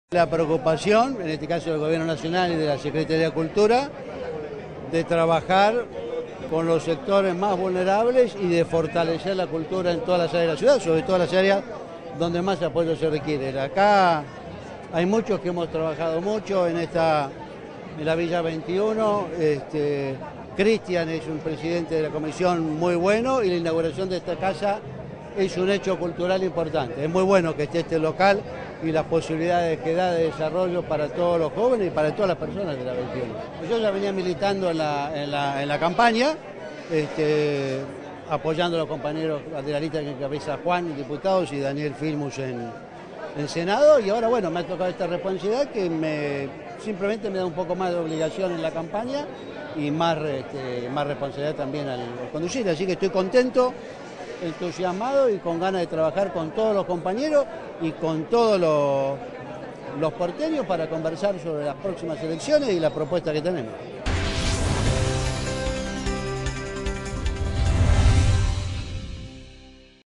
El candidato del FPV, Taiana, participó de la inauguración de la Casa de la Cultura Villa 21
Jorge Taiana, primer candidato a legislador porteño por el Frente para la Victoria (FpV), se expresó optimista respecto a la próxima elección en la Ciudad de Buenos Aires, al participar de la inauguración de la "Casa de la Cultura Villa21" en el barrio porteño de Barracas.

Durante el acto, el ex canciller acompaño a la Presidenta Cristina Fernández de Kirchner junto a los candidatos a senador nacional y diputado del Frente para la Victoria, Daniel Filmus y Juan Cabandié; Pablo Ferreyra, quien encabeza la colectora Alternativa Popular y es hermano de Mariano, el militante del PO asesinado; y el ministro de Trabajo, Carlos Tomada.

La Casa de la Cultura Villa 21, en el barrio de Barracas, contará con un auditorio para 300 personas, aulas, talleres y un canal de televisión.